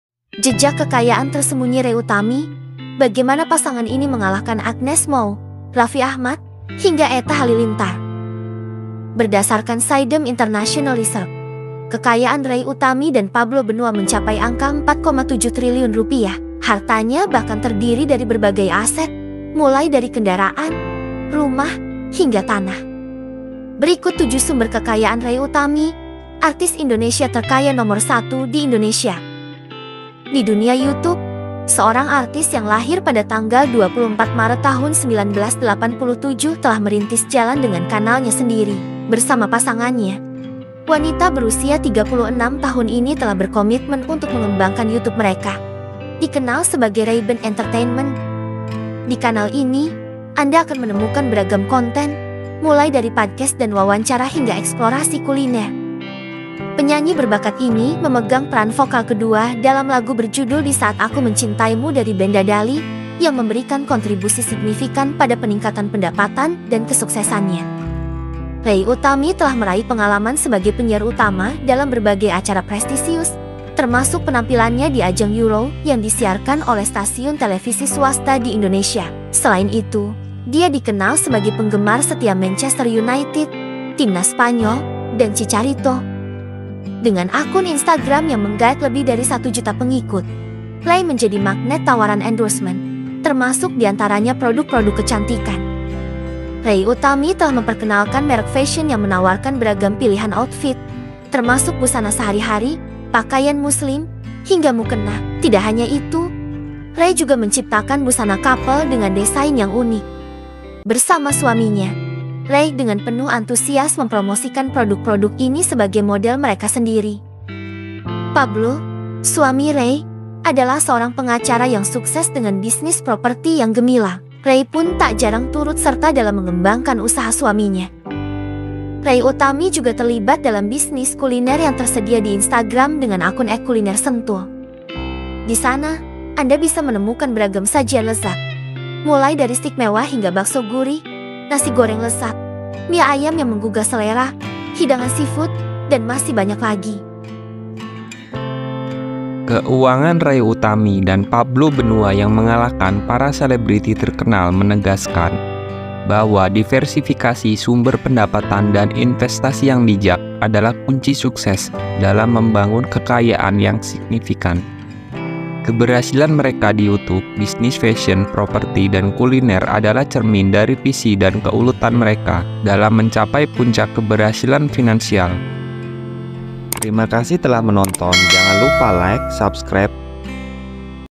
Jelajahi Sumber Kekayaan Rey Utami, Artis Indonesia Terkaya #1 🔥
Dalam video ini, kami akan mengungkap 7 sumber kekayaan Rey Utami dan Pablo Benua yang mencapai Rp4,7 triliun !
Yuk, simak kisah sukses pasangan ini !